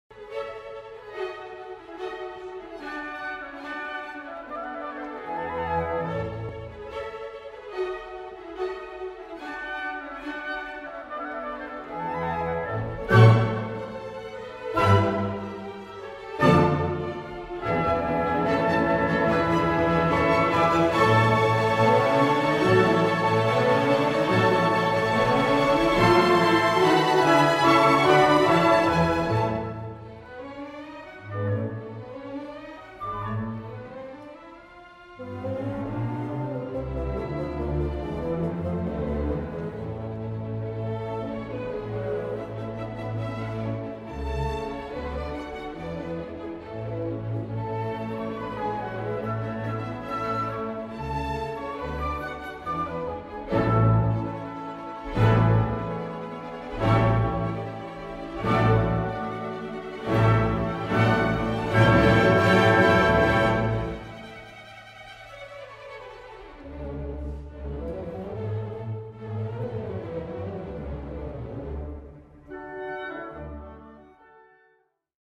AYO Matters to Us | Support AYO Today
It has been heartening to receive your messages and support so far, and to see the different ways AYO has impacted the lives of so many young musicians, professional artists and audiences around the country. In this video, we share some of these personal reflections.